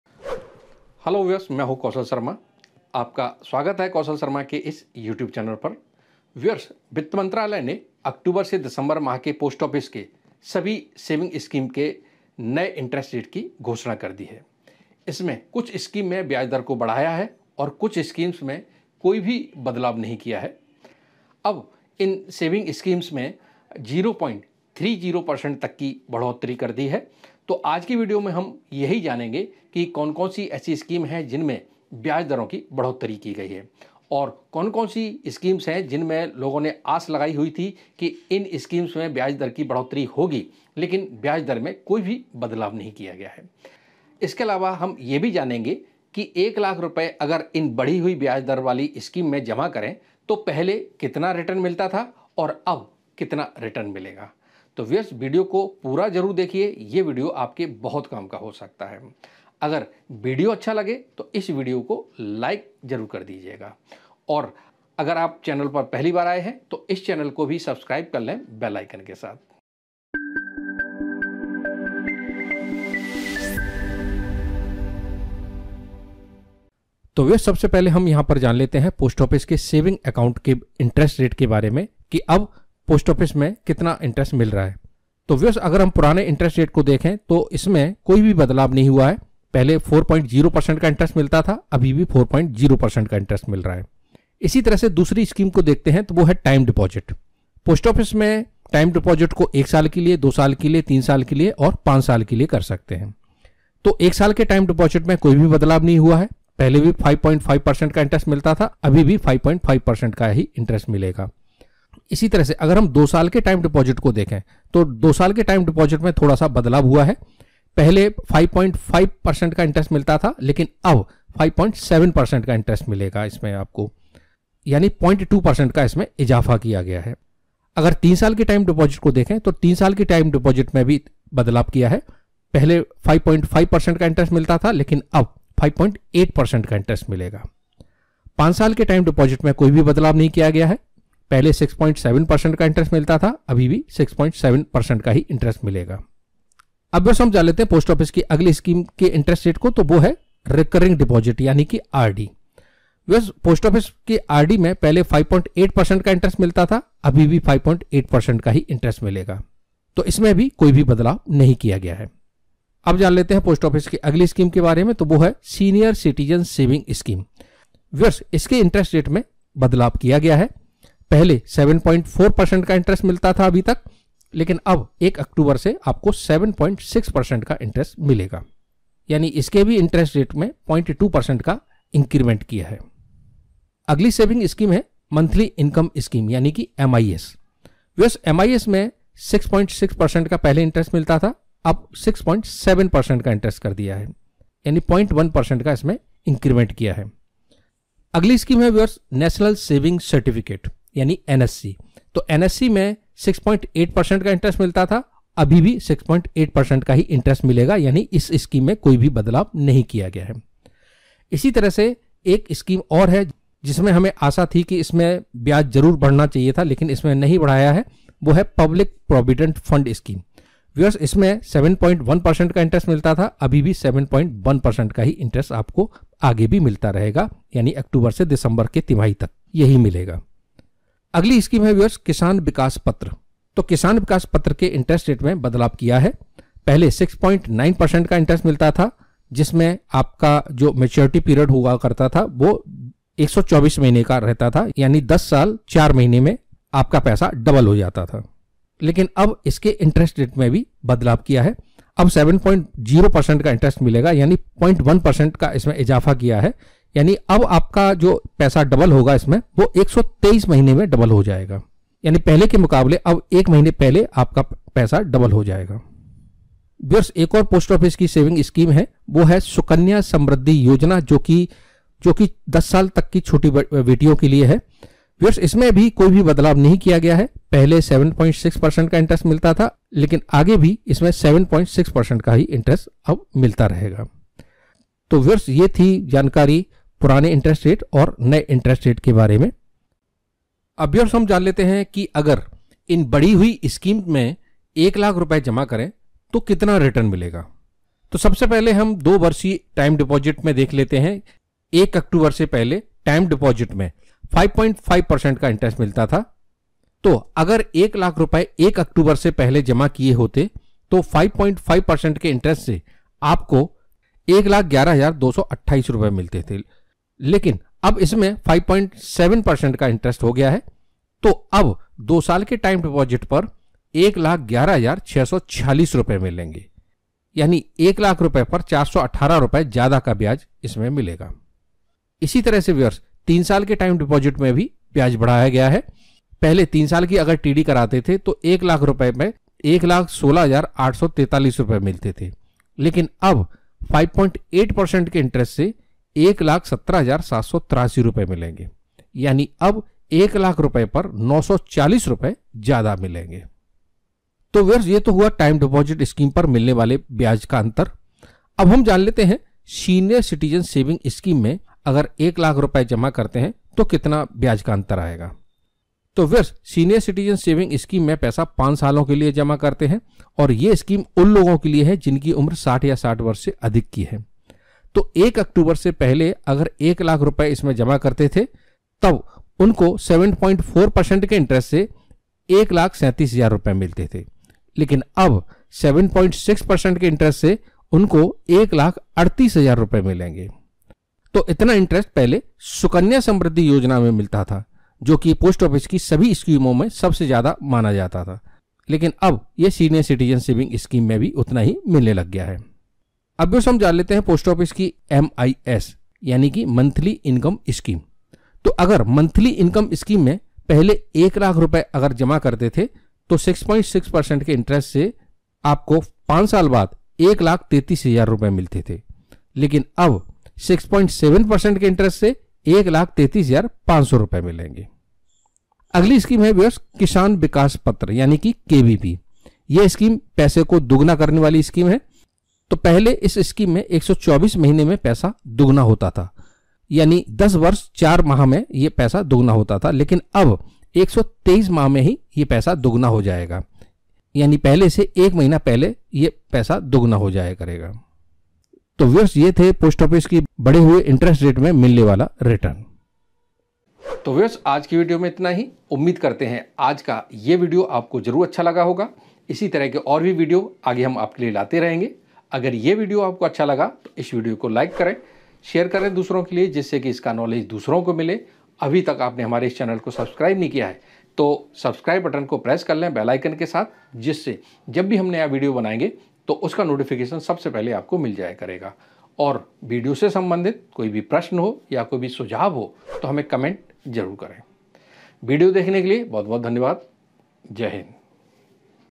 पोस्ट ऑफिस बचत योजनाओ की नई ब्याज दरें | Post Office New Interest Rates October 2022 | PO new Rates
पोस्ट ऑफिस की सभी बचत योजनाओ की Octobet से December 2022 के नए ब्याज दरों की घोषणा हो चुकी है, जानिए अब क्या हो गईं हैं नई ब्याज दरें?
तो वीडियो को पूरा देखें ये आपके लिए बहुत उपयोगी हो सकता है.

► TOPIC KEYWORDS
Kaushal Sharma
Computer K Videos